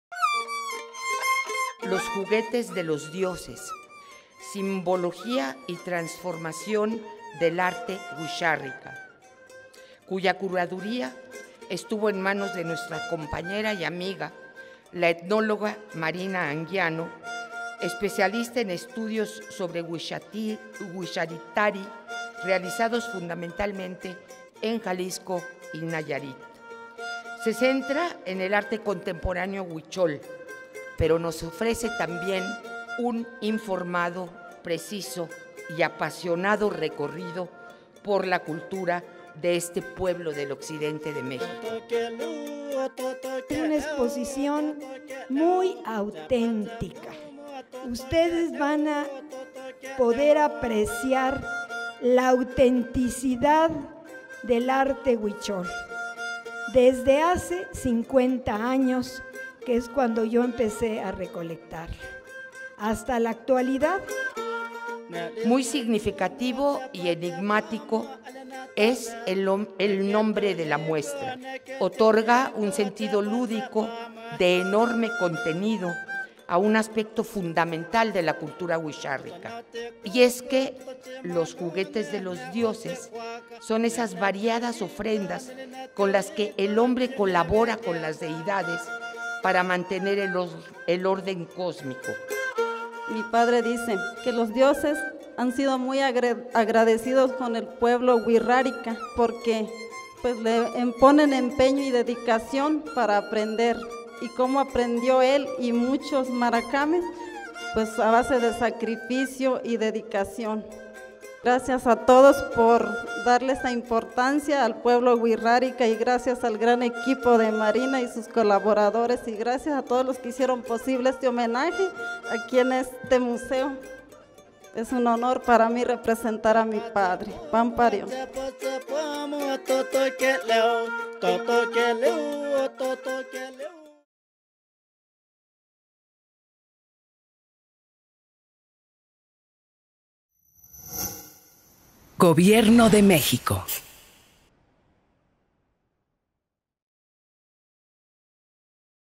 Inauguración "Los juguetes de los dioses: simbología y transformación del arte wixárika"
Momentos de la inauguración de la Exposición temporal "Los juguetes de los dioses: simbología y transformación del arte wixárika o huichol. Homenaje al mara´akáme y artista Eligio Carrillo Vicente", que se realizó el pasado jueves 12 de septiembre.